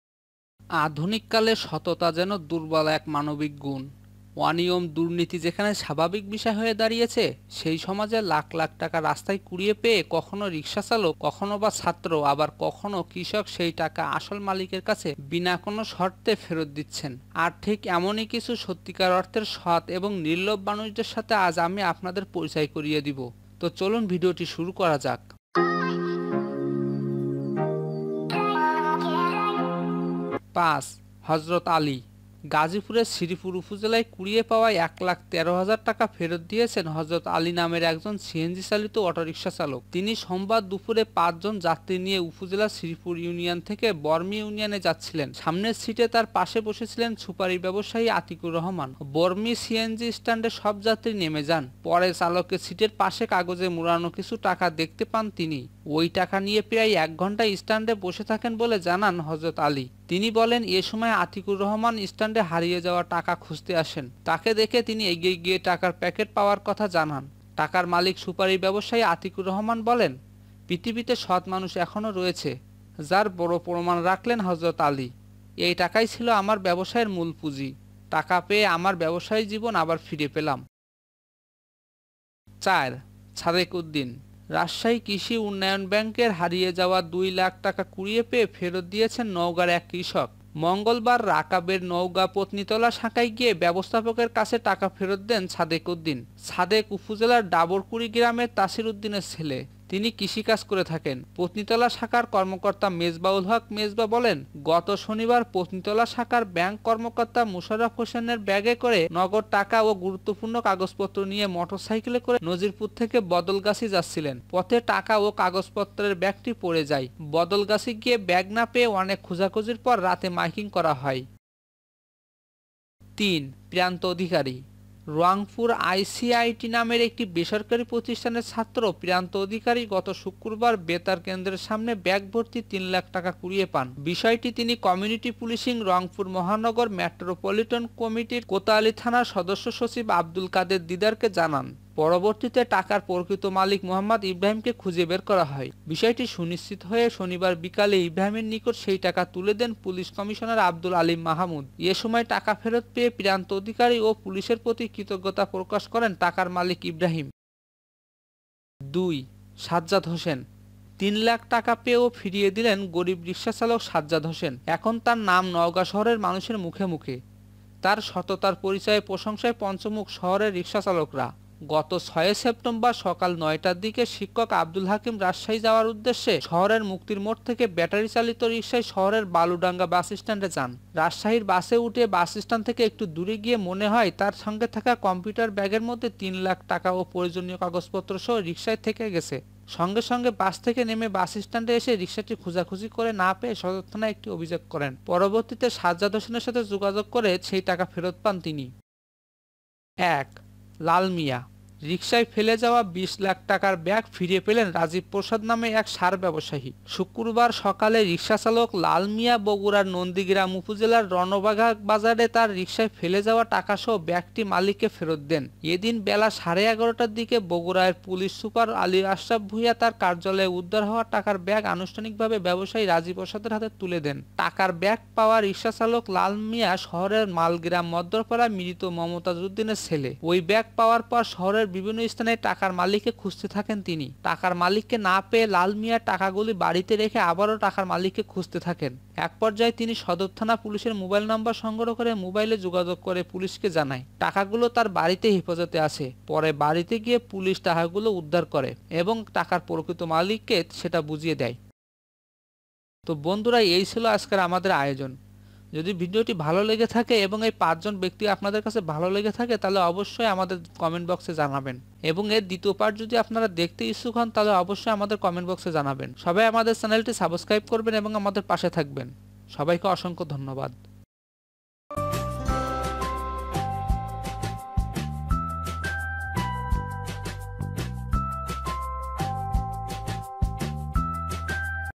বাংলাদেশের সবচেয়ে সেরা ৫জন সৎ ব্যক্তি ।। Top 5 Best Honest People in Bangladesh
আধুনিককালে সততা যেন দুর্বল এক মানবিক গুণ। অনিয়ম-দুর্নিতি যেখানে স্বাভাবিক বিষয় হয়ে দাড়িয়েছে, সেই সমাজে লাখ লাখ টাকা রাস্তায় কুড়িয়ে পেয়ে কখনও রিকশাচালক কখনো বা ছাত্র আবার কখনো কৃষক সেই টাকা আসল মালিকের কাছে বিনা কোন শর্তে ফেরত দিচ্ছেন। আর ঠিক এমনি কিছু সত্যিকার অর্থের সৎ এবং নির্লোভ মানুষদের সাথে আজ আমি আপনাদের পরিচয় করে দিবো। তো চলুন ভিডিও শুরু করি।

১। লাল মিয়া
রিকশায় ফেলে যাওয়া ২০ লাখ টাকার ব্যাগ ফিরে পেলেন রাজীব প্রসাদ (৩৫) নামে এক সার ব্যবসায়ী। শুক্রবার সকালে রিকশাচালক লাল মিয়া (৫৫) বগুড়ার নন্দীগ্রাম উপজেলার রনবাঘা বাজারে তার রিকশায় ফেলে যাওয়া টাকাসহ ব্যাগটি মালিককে ফেরত দেন। এদিন বেলা সাড়ে ১১টার দিকে বগুড়ার পুলিশ সুপার আলী আশরাফ ভূঞা তার কার্যালয়ে উদ্ধার হওয়া টাকার ব্যাগ আনুষ্ঠানিকভাবে ব্যবসায়ী রাজীব প্রসাদের হাতে তুলে দেন। টাকার ব্যাগ পাওয়া রিকশাচালক লাল মিয়া শহরের মালগ্রাম মধ্যপাড়ার মৃত মমতাজ উদ্দিনের ছেলে। ওই ব্যাগ পাওয়ার পর শহরের বিভিন্ন স্থানে টাকার মালিককে খুঁজতে থাকেন তিনি। টাকার মালিককে না পেয়ে লাল মিয়া টাকাগুলো বাড়িতে রেখে আবারো টাকার মালিককে খুঁজতে থাকেন। এক পর্যায়ে তিনি সদর থানা পুলিশের মোবাইল নম্বর সংগ্রহ করে মোবাইলে যোগাযোগ করে পুলিশকে জানায়, টাকাগুলো তার বাড়িতে হেফাজতে আছে। পরে বাড়িতে গিয়ে পুলিশ টাকাগুলো উদ্ধার করে।

২। সাজ্জাদ হোসেন
তিন লাখ টাকা পেয়েও ফিরিয়ে দিলেন গরিব রিকশাচালক সাজ্জাদ হোসেন। এখন তার নাম নওগাঁ শহরের মানুষের মুখে মুখে। তার সততার পরিচয়ে প্রশংসায় পঞ্চমুখ শহরের রিকশাচালকরা। গত ৬ সেপ্টেম্বর সকাল ৯টার দিকে শিক্ষক আবদুল হাকিম রাজশাহী যাওয়ার উদেশে শহরের মুক্তির মোড় থেকে ব্যাটারিচালিত রিকশায় শহরের বালুডাঙা বাসস্ট্যান্ড যান। রাজশাহীর বাসে উঠে বাসস্ট্যান্ড থেকে একটু দূরে গিয়ে মনে হয়, তার সঙ্গে থাকা কম্পিউটার ব্যাগের মধ্যে তিন লাখ টাকা ও প্রয়োজনীয় কাগজপত্রসহ রিকশায় থেকে গেছে।সঙ্গে সঙ্গে বাস থেকে নেমে বাসস্ট্যান্ডে এসে রিকশাটি খোঁজাখুঁজি করে না পেয়ে সদর থানায় একটি অভিযোগ করেন।

৩। প্রান্ত অধিকারী
রংপুর আরসিআইটি নামের একটি বেসরকারি প্রতিষ্ঠানের ছাত্র প্রান্ত অধিকারী গত শুক্রবার বেতার কেন্দ্রের সামনে ব্যাগ ভর্তি ৩ লাখ টাকা কুড়িয়ে পান। বিষয়টি তিনি কমিউনিটি পুলিশিং রংপুর মহানগর মেট্রোপলিটন কমিটির কোতোয়ালী থানার সদস্য সচিব আব্দুল কাদের দিদারকে জানান। পরবর্তীতে টাকার প্রকৃত মালিক মো. ইব্রাহিমকে খুঁজে বের করা হয়।বিষয়টি সুনিশ্চিত হয়ে শনিবার বিকালে ইব্রাহিমের নিকট সেই টাকা তুলে দেন পুলিশ কমিশনার আব্দুল আলীম মাহমুদ।এ সময় টাকা ফেরত পেয়ে প্রান্ত অধিকারী ও পুলিশের প্রতি কৃতজ্ঞতা প্রকাশ করেন টাকার মালিক ইব্রাহিম।

৪। সাদেক উদ্দীন
রাজশাহী কৃষি উন্নয়ন ব্যাংকের (রাকাব) হারিয়ে পাওয়া দুই লাখ টাকা কুড়িয়ে পেয়ে ফেরত দিয়েছেন নওগাঁর এক কৃষক। মঙ্গলবার রাকাবের নওগাঁ পত্নীতলা শাখায় গিয়ে ব্যবস্থাপকের কাছে টাকা ফেরত দেন সাদেক উদ্দীন। সাদেক উপজেলার ডাবড়কুড়ি গ্রামের তাছির উদ্দীনের ছেলে। তিনি কৃষিকাজ করেন। পত্নীতলা শাখার কর্মকর্তা মেজবাউল হক মেজবা বলেন, গত শনিবার পত্নীতলা শাখার ব্যাংক কর্মকর্তা মোশাররফ হোসেন ব্যাগে করে নগদ লাখ টাকা ও গুরুত্বপূণ কাগজপত্র নিয়ে মোটরসাইকেলে করে নজিপুর থেকে বদলগাছী যাচ্ছিলেন। পথে টাকা ও কাগজপত্রের ব্যাগটি পড়ে যায়। বদলগাছী গিয়ে ব্যাগ না পেয়ে অনেক খোঁজাখুজির পর রাতে মাইকিং করা হয়।

৫। হযরত আলী
গাজিপুরের শ্রীপুর উপজেলায় কুড়িয়ে পাওয়া ১ লাখ ১৩ হাজার টাকা ফেরত দিয়েছেন হযরত আলী (৪৫) নামের একজন সিএনজিচালিত অটোরিকশাচালক। তিনি সোমবার দুপুরে পাঁচজন যাত্রী নিয়ে উপজেলার শ্রীপুর ইউনিয়ন থেকে বরমী ইউনয়নে যাচ্ছিলেন। সামনের সিটে তার পাশে বসেছিলেন সুপারি ব্যবসায়ী আতিকুর রহমান। বরমী সিএনজি স্ট্যান্ডে সব যাত্রী নেমে যান। পরে চালকের সিটের পাশে কাগজে মোড়ানো কিছু টাকা দেখতে পান তিনি। ওই টাকা নিয়ে প্রায় এক ঘণ্টা স্ট্যান্ডে বসে থাকেন বলে জানান হযরত আলী। তিনি বলেন, এ সময় আতিকুর রহমান স্ট্যান্ডে হারিয়ে যাওয়া টাকা খুঁজতে আসেন। তাকে দেখে তিনি এগিয়ে গিয়ে টাকার প্যাকেট পাওয়ার কথা জানান।টাকার মালিক সুপারি ব্যবসায়ী আতিকুর রহমান বলেন, "পৃথিবীতে সৎ মানুষ এখনো রয়েছে। যার প্রমাণ রাখলেন হযরত আলী। এ টাকাই ছিল আমার ব্যবসায়ের মূল পুঁজি। টাকা পেয়ে আমার ব্যবসায়ী জীবন ফিরে পেলাম।"